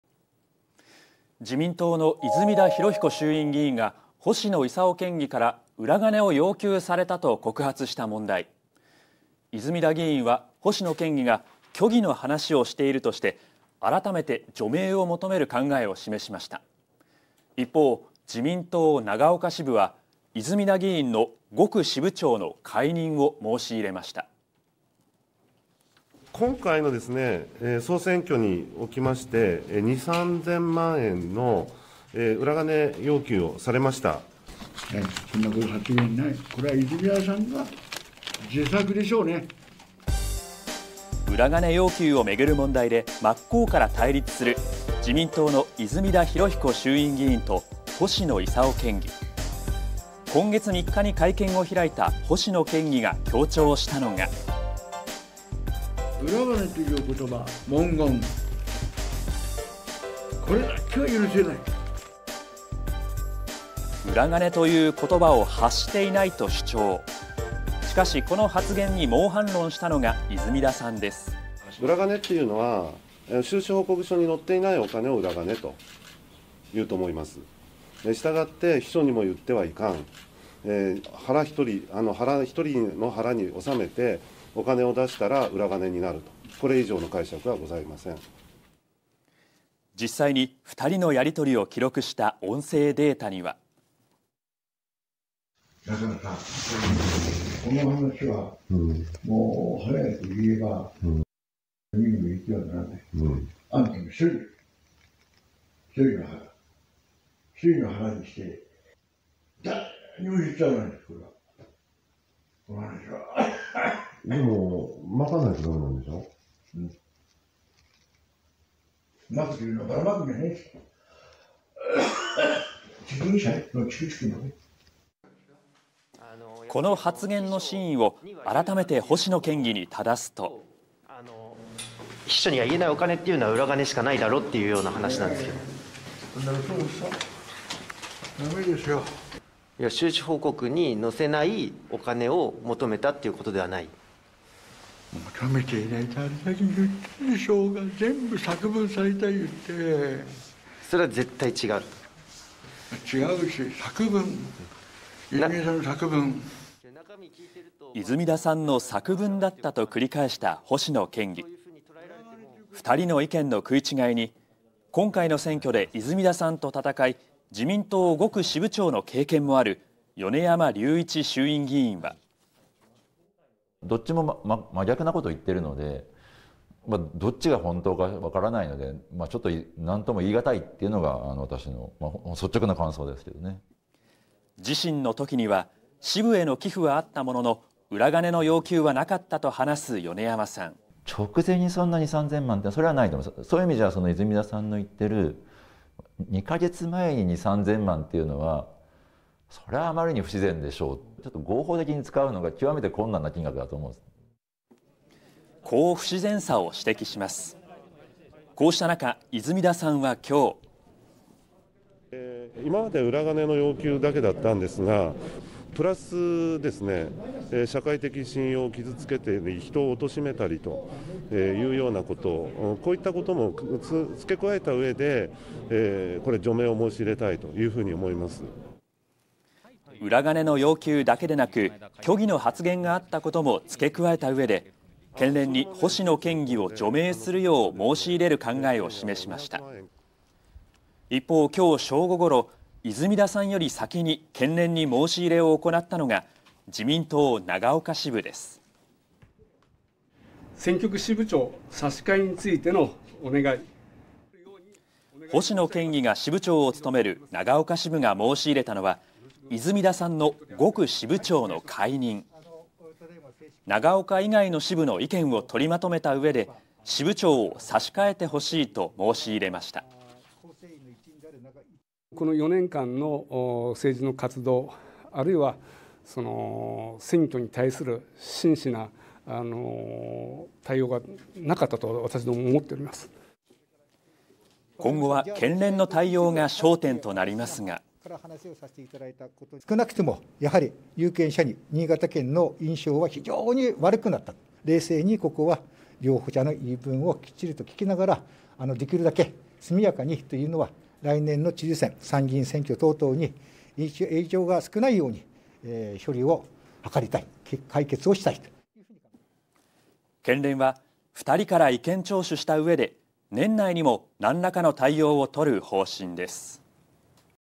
“裏金要求”めぐり… 応酬続く泉田議員と星野県議 長岡支部は５区支部長解任を申し入れ【新潟】 (21/12/06 19:13)
自民党の泉田裕彦 衆院議員が星野伊佐夫 新潟県議から「裏金を要求された」と告発した問題。

泉田議員は星野県議が虚偽の話しをしているとして、改めて除名を求める考えを示しました。一方、長岡支部は泉田議員の５区支部長の解任を申し入れました。

【泉田裕彦 衆院議員】
「今回の総選挙におきまして、２～３０００万円の裏金要求をされました」

【星野伊佐夫 県議】
「そんなこと、発言ない。これは泉田さんの自作でしょうね」

裏金要求をめぐる問題で、真っ向から対立する自民党の泉田裕彦衆院議員と星野伊佐夫県議。

３日に会見を開いた星野県議が強調したのが…

【星野伊佐夫 県議】
「裏金という言葉・文言、これだけは許せない」

“裏金”という言葉を発していないと主張。

しかし、この発言に猛反論したのが泉田さんです。

「裏金というのは収支報告書に載っていないお金を裏金というと思う。従って秘書にも言ってはいかん、一人の腹におさめてお金を出したら裏金になると、これ以上の解釈はございません」

実際に２人のやりとりを記録した音声データには…

《音声データ》
星野県議とされる人物：
「だからさ、この話しはもう早く言えば（秘書）の耳にも入れてはならない。あんた一人、一人の腹、だれにも言っちゃならない。これは、この話しは」

「でも、まかないとダメなんでしょ？」

「まくというのは、ばらまくんじゃない。実力者、地区地区の」

この発言の真意を改めて星野県議に質すと…

【星野伊佐夫 県議】
Ｑ．秘書には言えないお金は裏金しかない
「うそ、うそ、ダメですよ」
Ｑ．収支報告に載せられないお金では
「求めていないって言ったでしょ。全部作文されたと言って」
Ｑ．絶対違う
「違うし、作文。泉田さんの作文」

泉田さんの作文だったと繰り返した星野県議。

２人の意見の食い違いに、今回の選挙で泉田さんと戦い、自民党５区支部長の経験もある米山隆一衆院議員は…

【米山隆一 衆院議員】
「どっちも真逆なことを言っている。どっちが本当か分からないので、なんとも言いがたいというのが私の率直な感想」

支部への寄付はあったものの、裏金の要求はなかったと話す米山さん。

「直前に２～３０００万とか、それはないと思う。そういう意味では、泉田さんの言っている２か月前に２～３０００万というのは、あまりに不自然でしょうという感じはある。２～３０００万万使い切れない。合法的に使うのは極めて困難な金額」

こう不自然さを指摘します。

こうしたなか、泉田さんは６日…

「今までは裏金の要求だけだったが、プラス社会的信用を傷つけて人を貶めたりというようなこと。こういったことも付け加えたうえで、除名を申し入れたい」

裏金の要求だけでなく、虚偽の発言があったことも付け加えたうえで県連に星野県議を除名するよう申し入れる考えを示しました。

一方、正午ごろ泉田さんより先に県連に申し入れを行ったのが、自民党長岡支部です。

【五井文雄 長岡市議】
「選挙区支部長、差し替えについてのお願い」

星野県議が支部長を務める長岡支部が申し入れたのは、泉田さんの５区支部長の解任。

長岡以外の支部の意見を取りまとめたうえで、支部長を差し替えてほしいと申し入れました。

「この４年間の政治活動、あるいは選挙に対する真摯な対応がなかったと私ども思っている」

今後は県連の対応が焦点となりますが…

【自民党県連　小野峯生 幹事長】
「少なくとも有権者に新潟県の印象は非常に悪くなったと、我々も苦慮しているということで戸惑っている。冷静に両者の言い分を聞きながら、できるだけ速やかにというのは、来年の知事選・参院選に影響が少ないように処理を図りたい。解決をしたい」

県連は２人から意見聴取したうえで、年内にも何らかの対応をとる方針です。